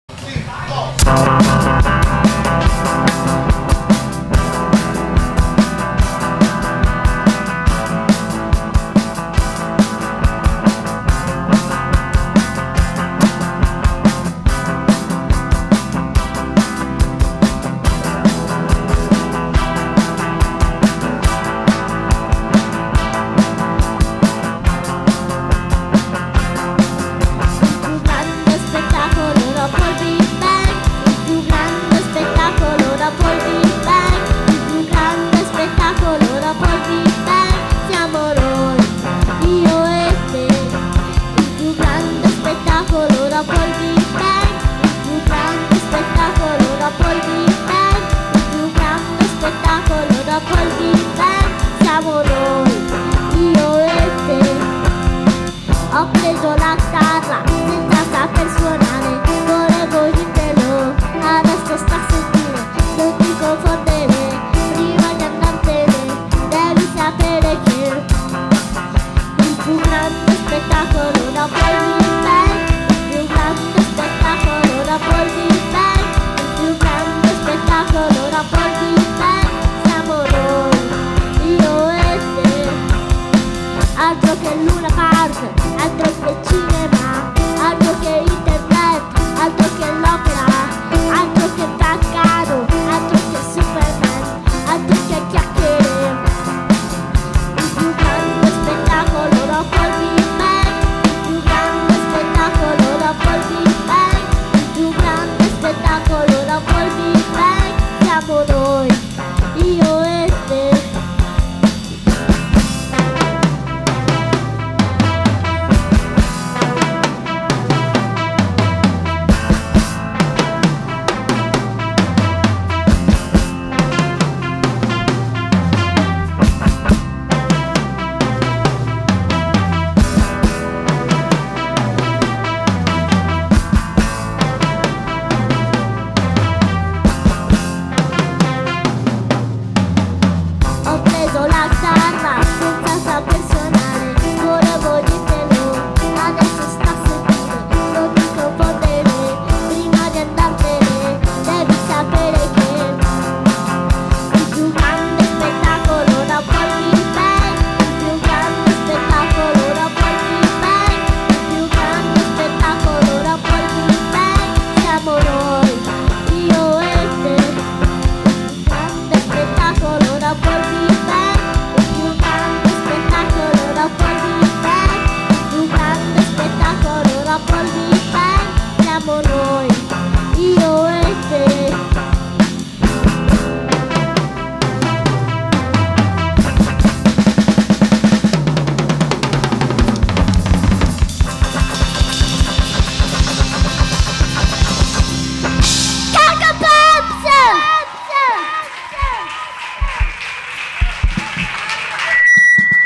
La Semicroma
I piccolissimi Coco Pops Live al Molo 5
interpretano Jovanotti
Marghera 6 Giugno 2014